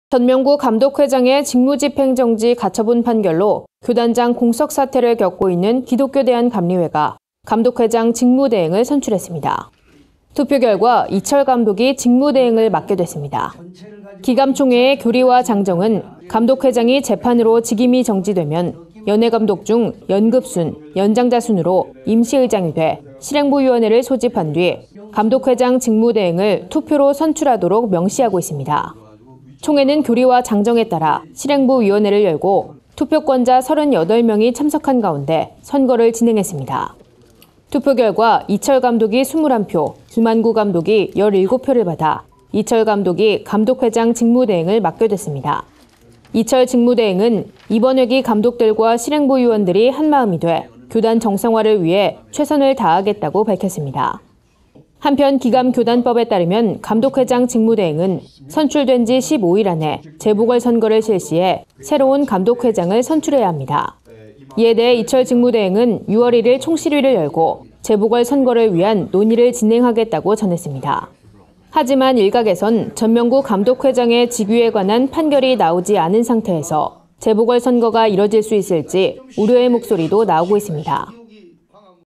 [GOODTV NEWS 20180521] 기감, 이철 감독 직무대행 선출
전명구 감독회장의 직무집행정지 가처분 판결로 교단장 공석사태를 겪고 있는 기독교대한감리회가 감독회장 직무대행을 선출했습니다. 투표 결과 이철 감독이 직무대행을 맡게 됐습니다.

기감 총회의 '교리와 장정'은 '감독회장이 재판으로 직임이 정지되면 연회 감독 중 연급순, 연장자 순으로 임시의장이 돼 실행부위원회를 소집한 뒤, 감독회장 직무대행을 투표로 선출하도록 명시하고 있습니다.

총회는 교리와 장정에 따라 실행부위원회를 열고 투표권자 38명이 참석한 가운데 감독회장 직무대행 선거를 진행했습니다. 투표 결과, 이철 감독이 21표, 김한구 감독이 17표를 받아 이철 감독이 감독회장 직무대행을 맡게 됐습니다.

이철 직무대행은 "이번 회기 감독들과 실행부위원들이 한 마음이 돼 교단 정상화를 위해 최선을 다하겠다"고 밝혔습니다.

한편 기감 교단법에 따르면 감독회장 직무대행은 선출된 지 15일 안에 재보궐선거를 실시해 새로운 감독회장을 선출해야 합니다. 이에 대해 이철 직무대행은 내달 1일 총실위를 열고 재보궐 선거를 위한 논의를 진행하겠다고 전했습니다.

하지만 일각에선 전명구 감독회장의 직위에 관한 판결이 나오지 않은 상태에서 재보궐 선거가 이뤄질 수 있을지 우려의 목소리도 나오고 있습니다.

* 시청방법
- 위성TV: KT 스카이라이프 193번